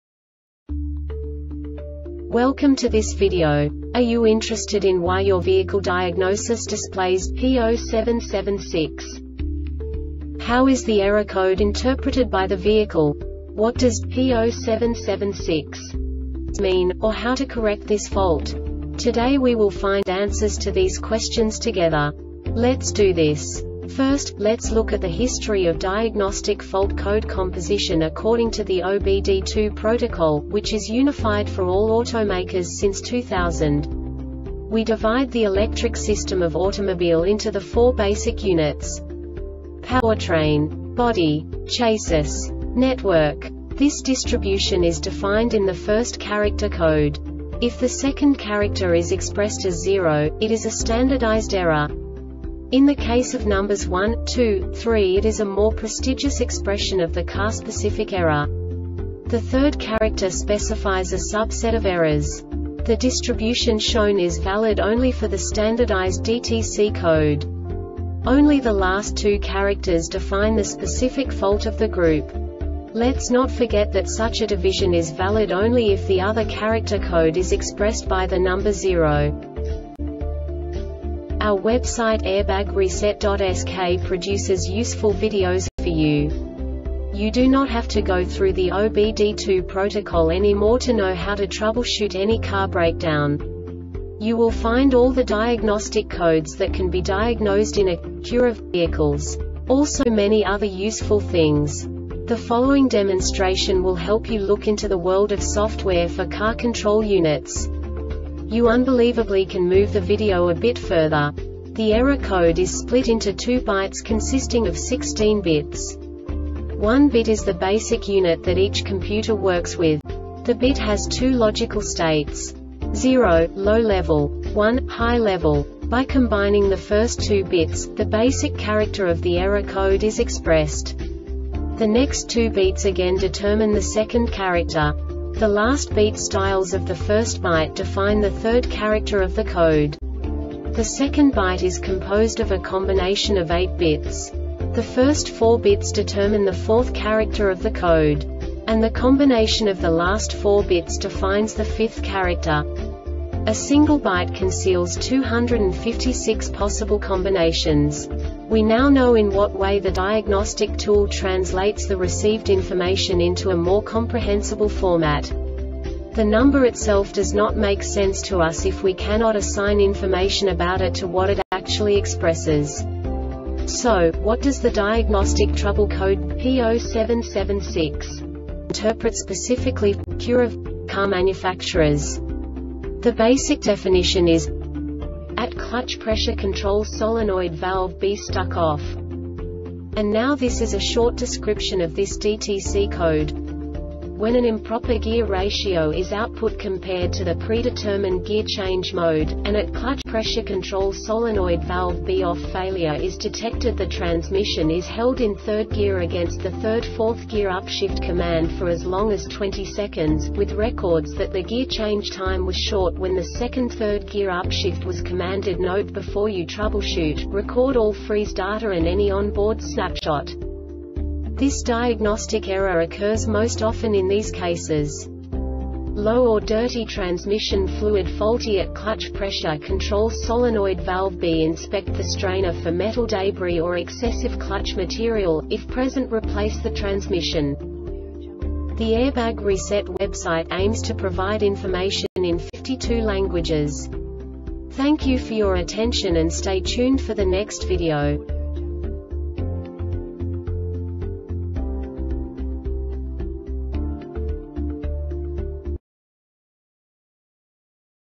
DTC Acura P0776 Short Explanation
Make:
Acura
Code:
P0776
Definition:
A/T Clutch Pressure Control Solenoid Valve B Stuck OFF
Description:
When an improper gear ratio is output compared to the predetermined gear change mode, an A/T clutch pressure control solenoid valve B OFF failure is detected. The transmission is held in 3rd gear against the 3rd-4th gear upshift command for as long as 20 seconds, with records that the gear change time was short when the 2nd-3rd gear upshift was commanded. NOTE: Before you troubleshoot, record all freeze data and any on-board snapshot.
Cause:
Low or dirty transmission fluid
Faulty A/T clutch pressure control solenoid valve B
Inspect the strainer for metal debris or excessive clutch material, if present
replace the transmission